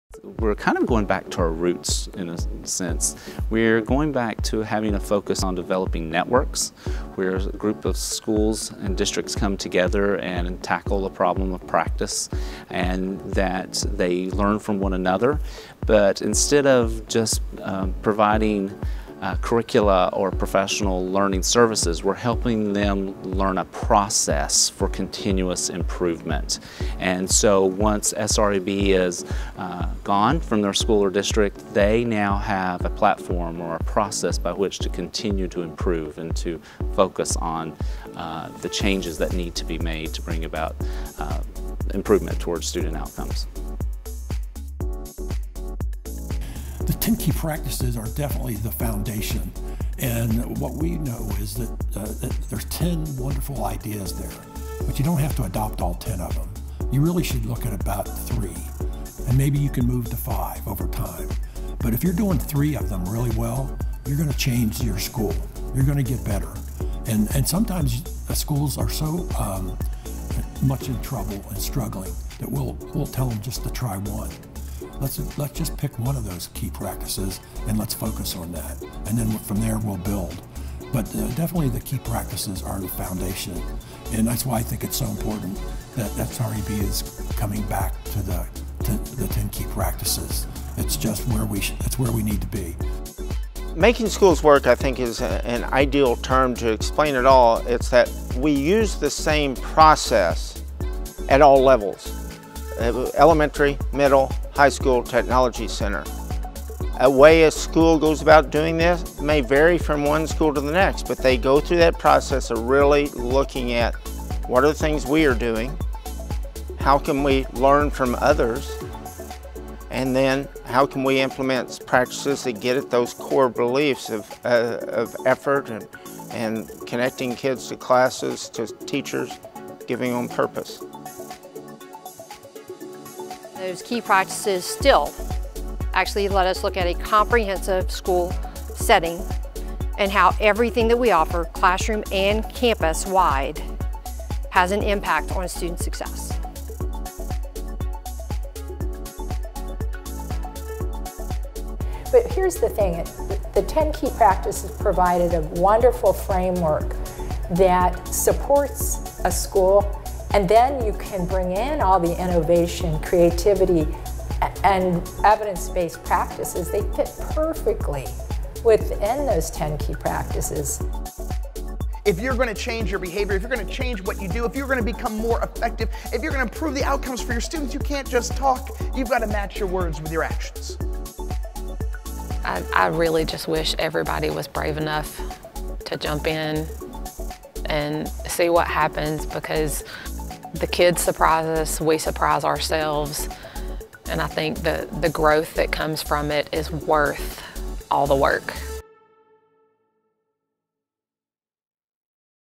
SREB School Improvement: Who We Are
In this video, SREB staff and state, district and school leaders and teachers share how SREB’s office of School Improvement partners with states, districts and schools to help teachers, counselors and principals shift their instructional practices from good to great. Hear how the research-based Key Practices of our Making Schools Work school improvement process empower students to make the effort needed to succeed.